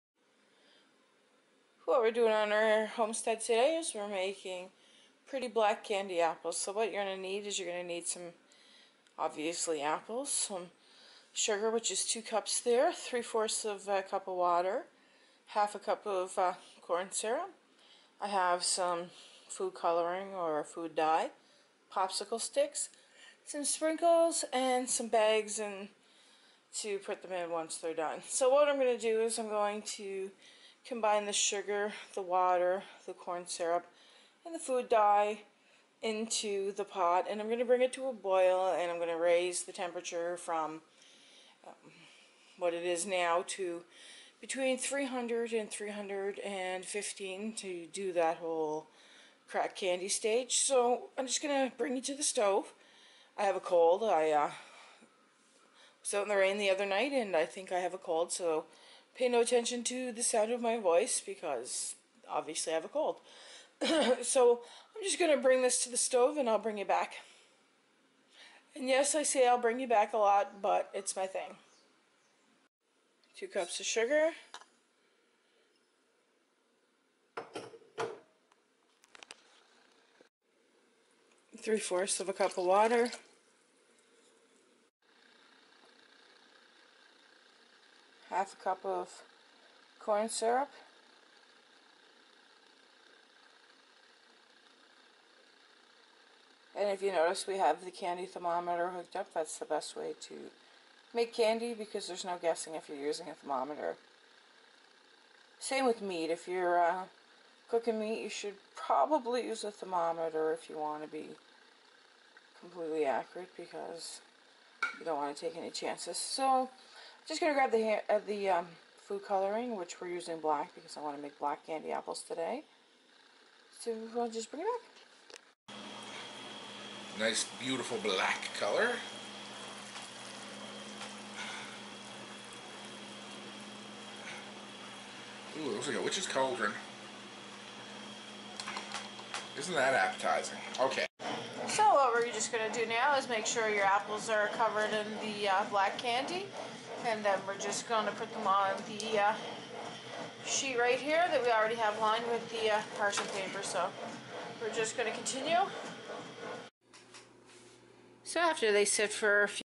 How To Make The Perfect Black Candy Apples For Halloween
In this video I show How to make easy black candy apples using ,
3/4 cup water
2 cups of sugar
1/2 cup of corn syrup
Combine in a pot boil these things until candy tem reachs 310 -315
then add in black food color ,, cover apples, if you want blood effect like the picture you need to make second batch of red , using red instead of black then use spoon and drizzle to give dripping blood look , enjoy and subscribe